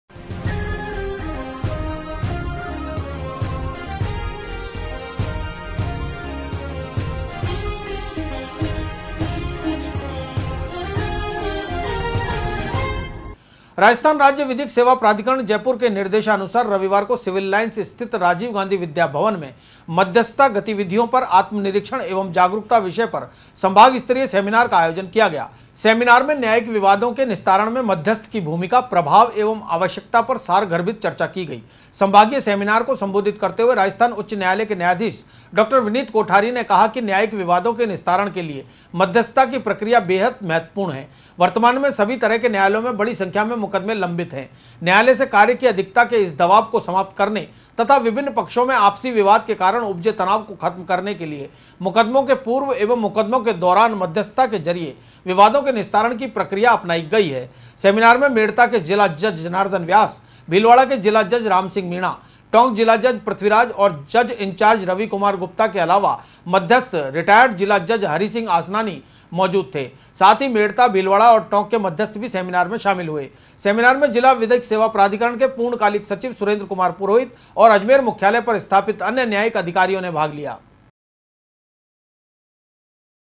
राज्य विधिक सेवा प्राधिकरण का सम्भाग स्तरीय सेमिनार
राज्य विधिक सेवा प्राधिकरण का सम्भाग स्तरीय सेमिनार मध्यस्थता गतिविधियों पर आत्मनिरीक्षण एवं जागरूकता विषय पर दिया मार्ग दर्शन।